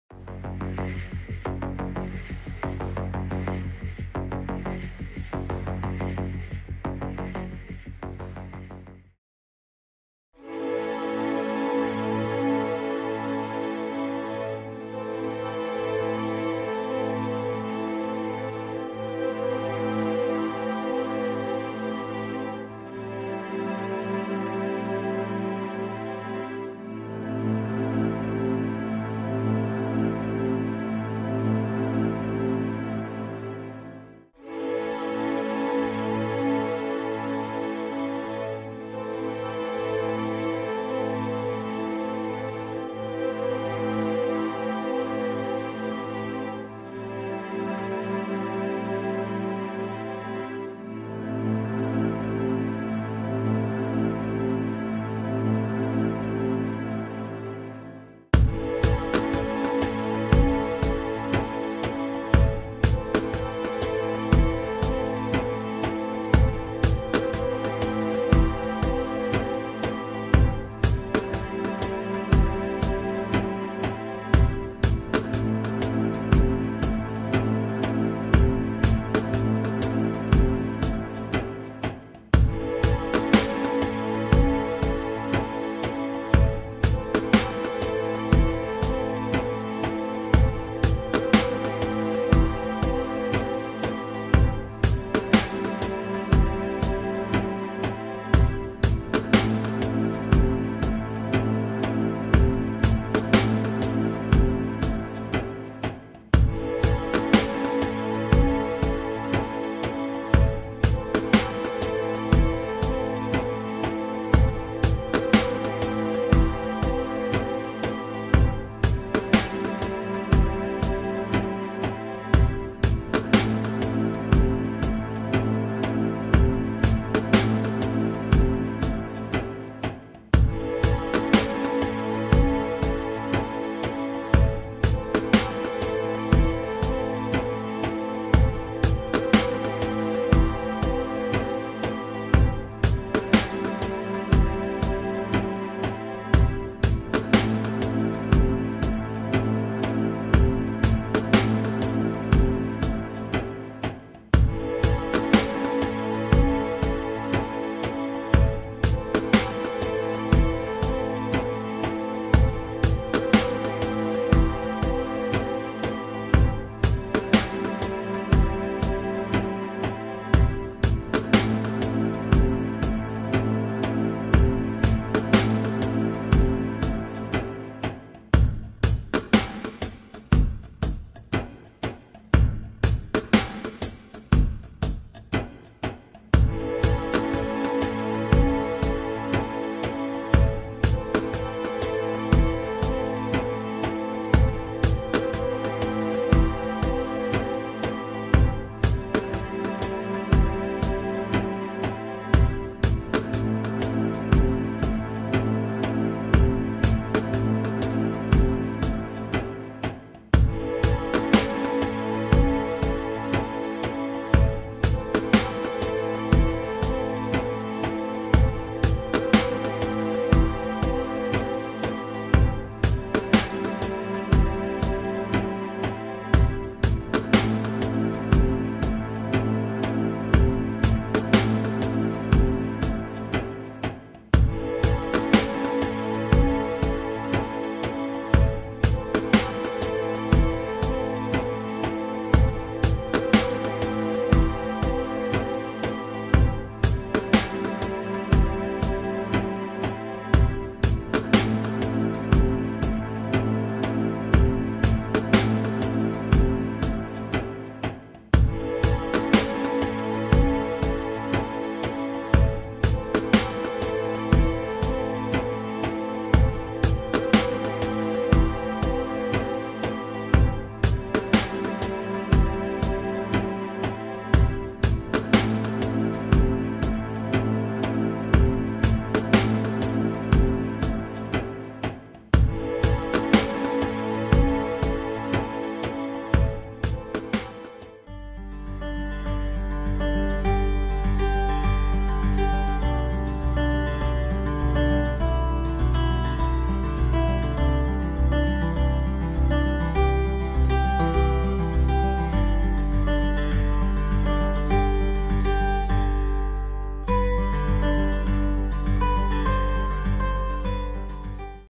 SkyArt Aerial - 1st Demo Reel
a combination of various shots around the twin cities. FAA Certified Commercial Pilot